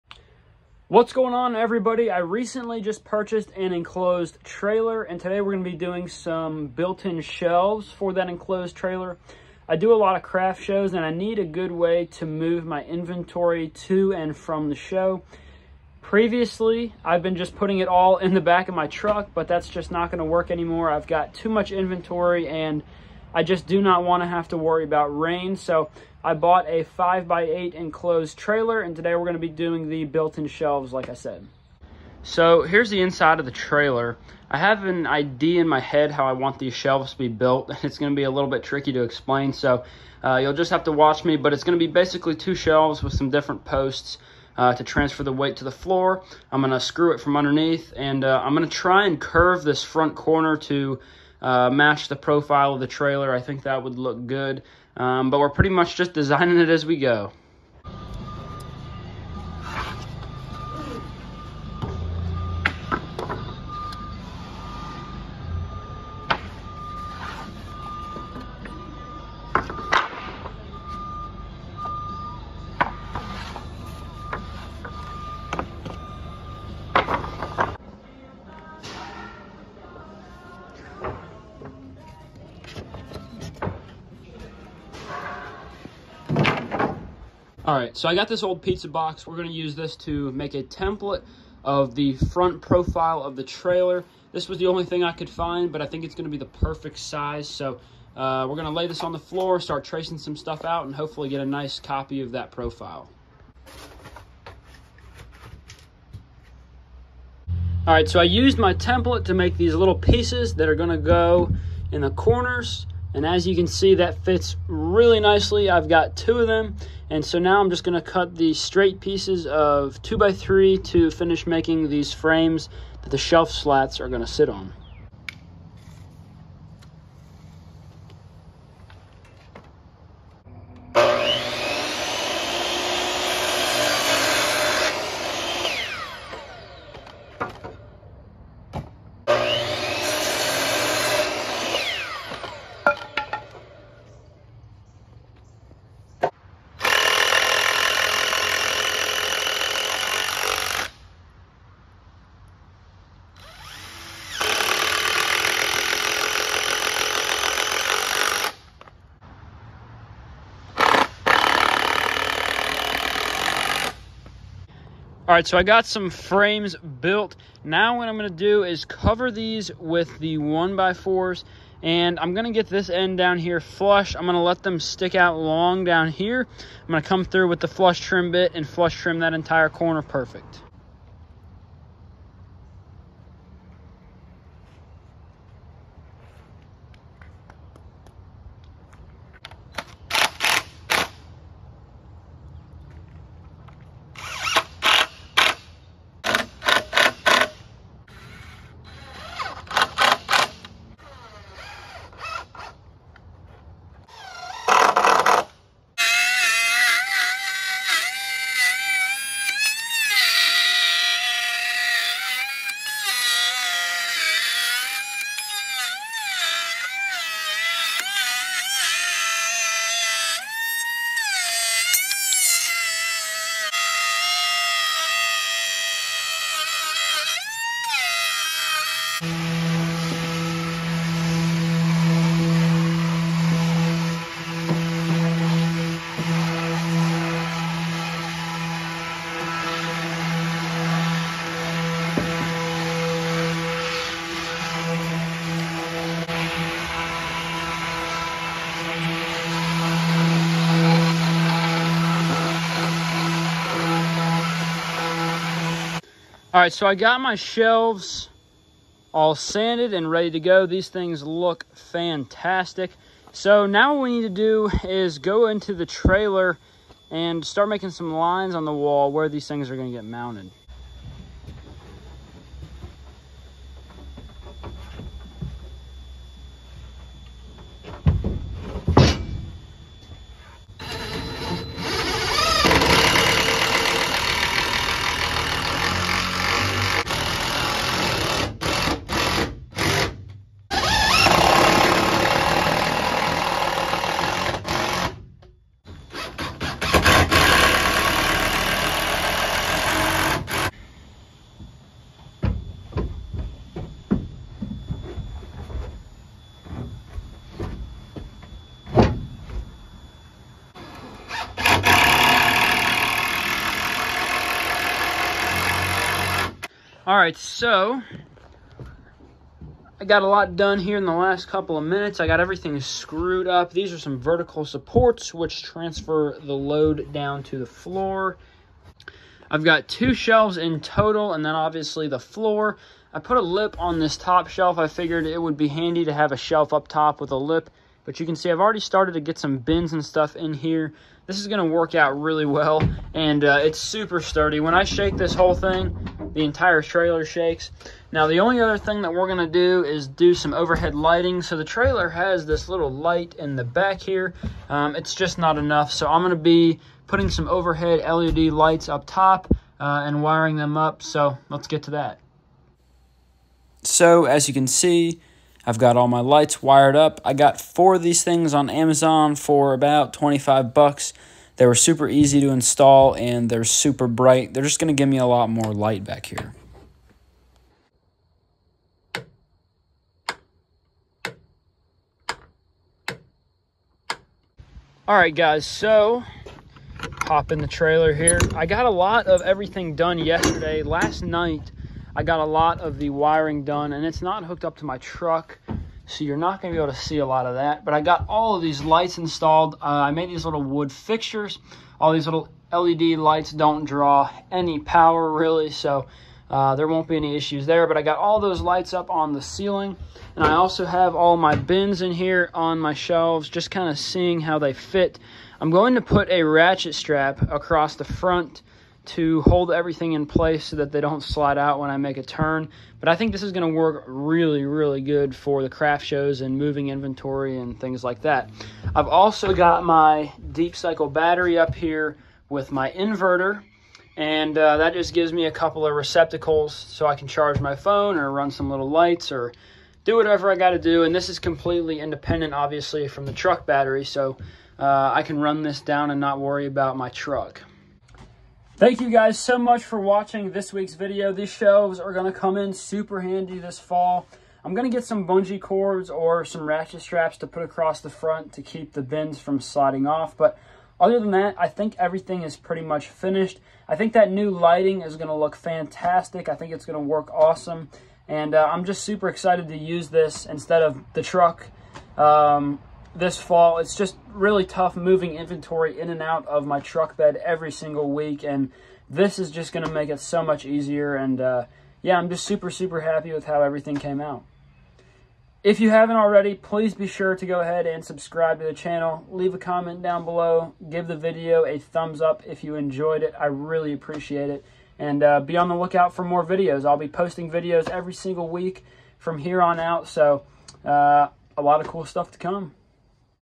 Building Shelves In an Enclosed Trailer
These shelves are gonna be a game changer for me. Like I mentioned in the video I have previously had to store all of my bins and inventory in the basement and carry them up each week to pack them in my truck. This will allow me to keep all of my inventory in one place and keep me from having to handle it so much. Still some minor things to do like a strap across the front and whatnot but the trailer is pretty close to the way I want it!